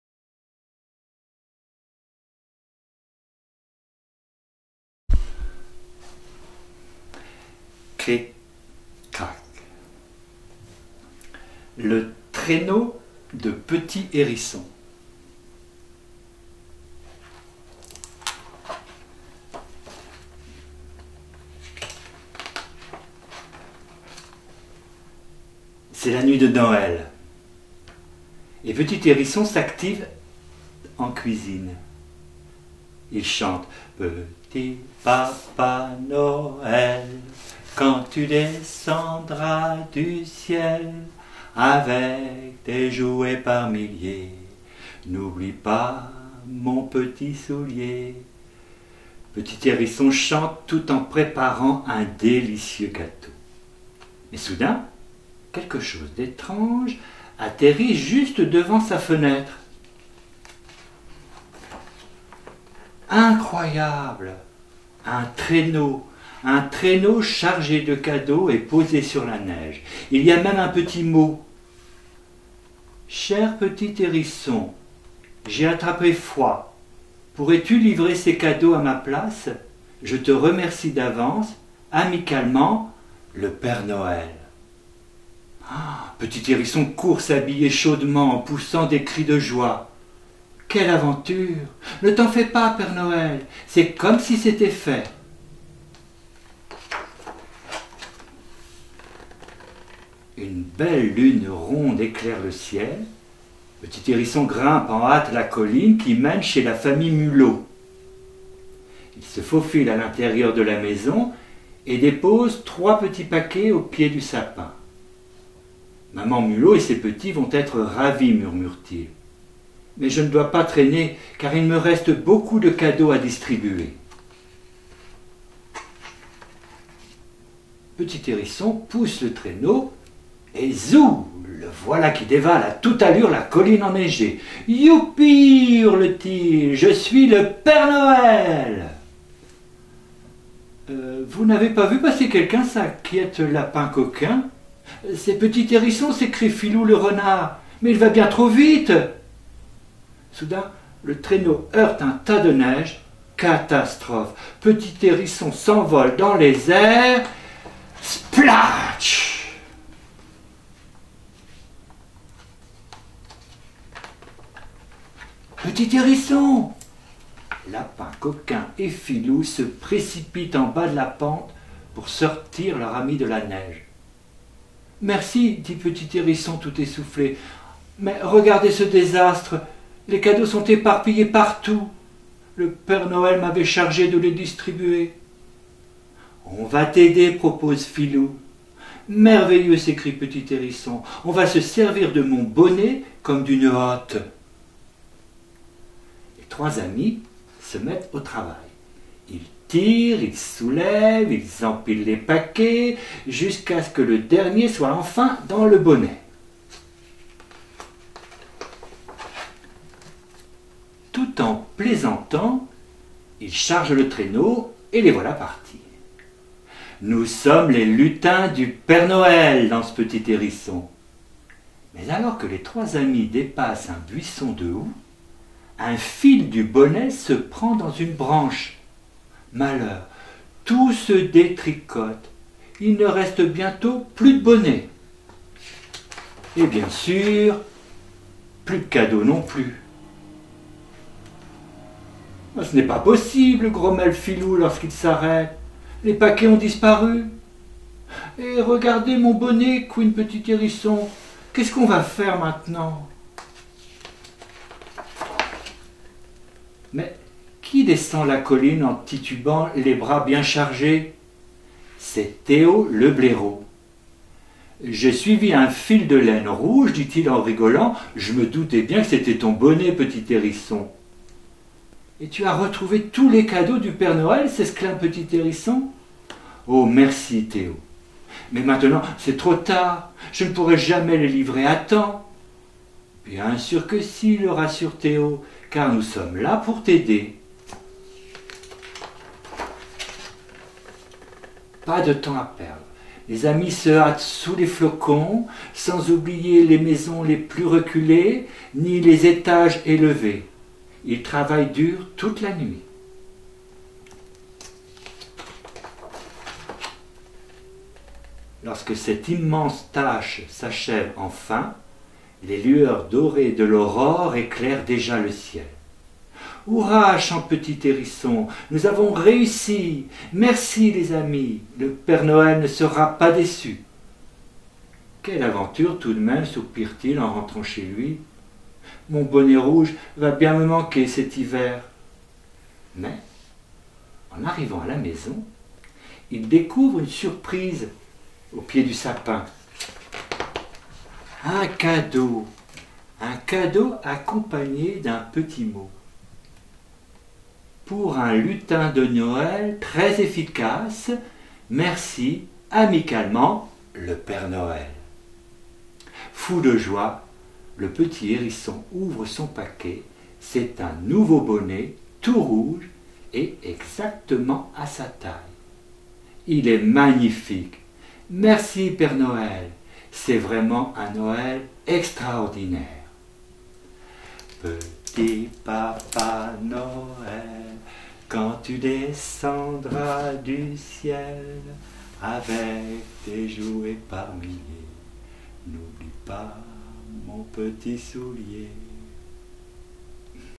Le traîneau de petit hérisson
Eric nous raconte l'histoire du traîneau du petit hérisson